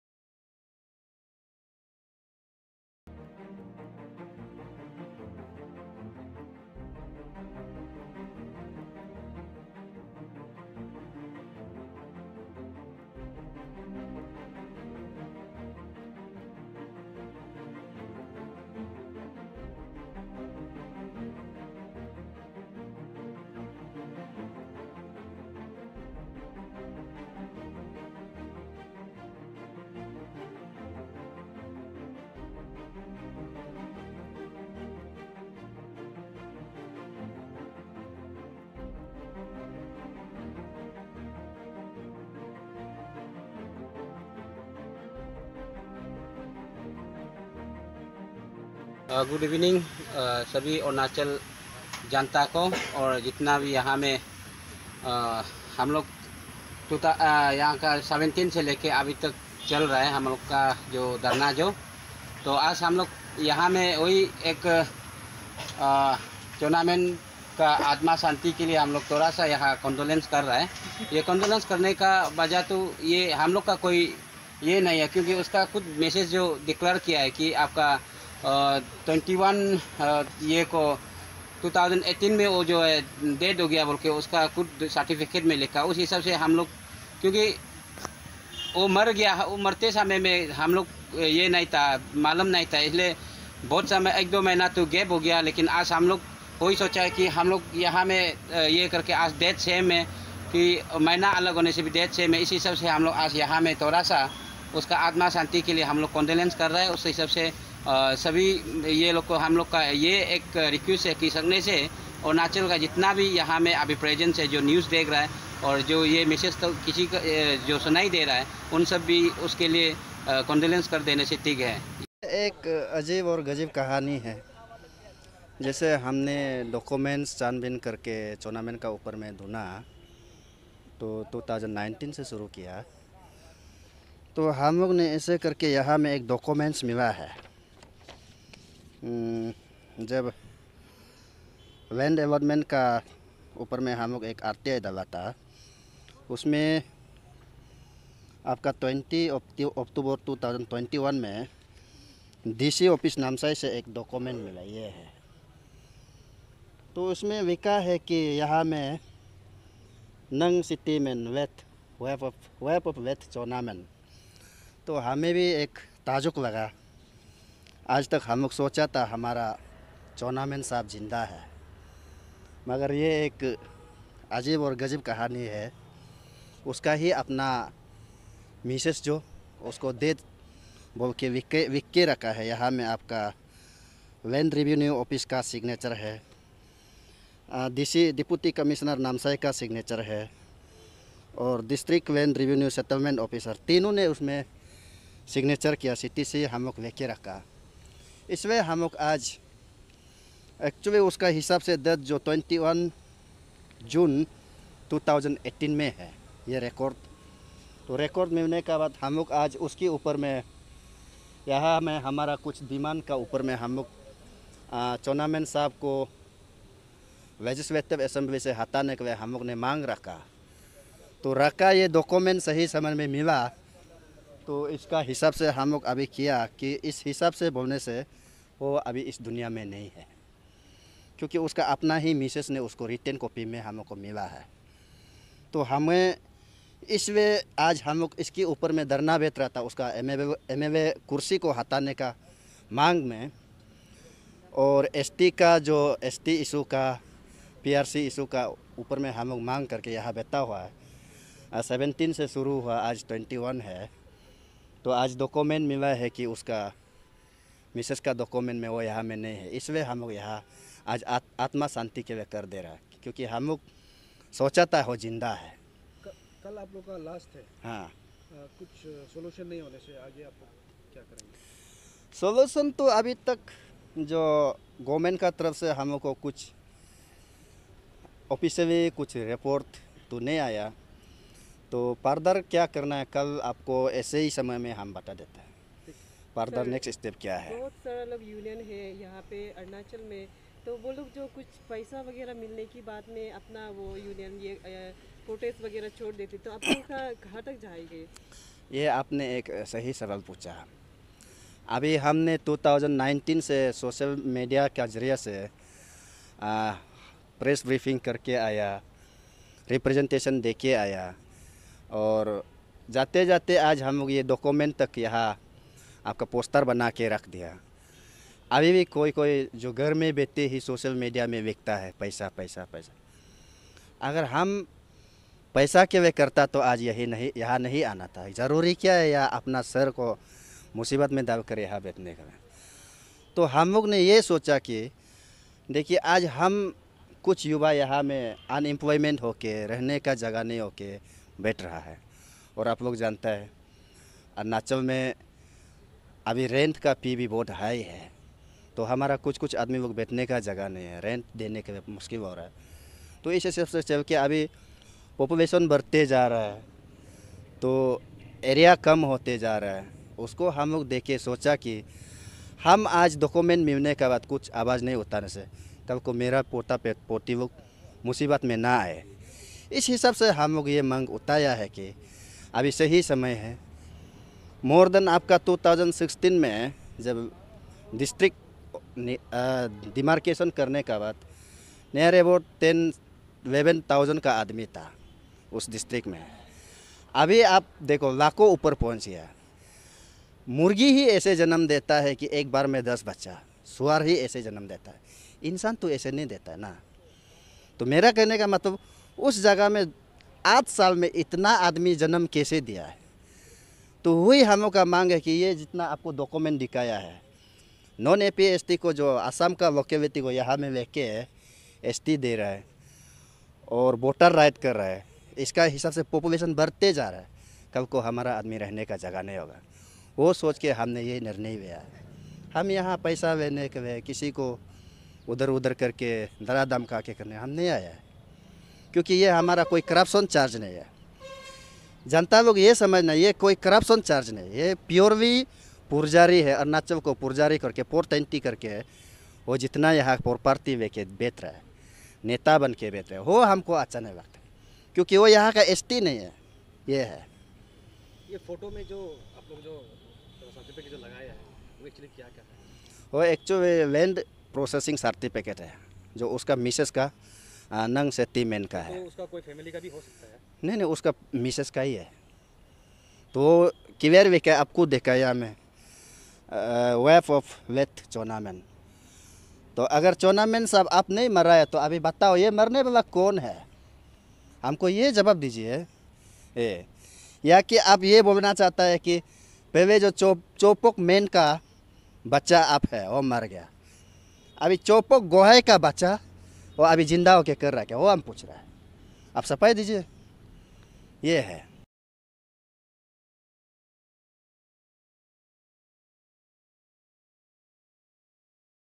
Today is the 4th day peaceful dharna of the All Arunachal Tribal Rural Panchayati
Today is the 4th day peaceful dharna of the All Arunachal Tribal Rural Panchayati Restoration Andolan Committee (AATRPRAC). The charter of demand consists of five main points.  The James Teli camdir General Secratary (AATRPRAC) informed to the media that, one of their charter demands is to remove Chowna Mein from the post of MLA. They filed RTI against Land Allotment. With this RTI, the most shocking information has been rolled out from DC Office, Namsai. According to the Land Possession Certificate, the NG Sati Mein, wife of Chawna Mein mentioned him as dead by mentioning Late before his husband’s name. Therefore, the AATRPRAC are giving condolences to the late Chowna Mein (Sarcastic). The further information’s are yet to be known.
Let's have a look at this report.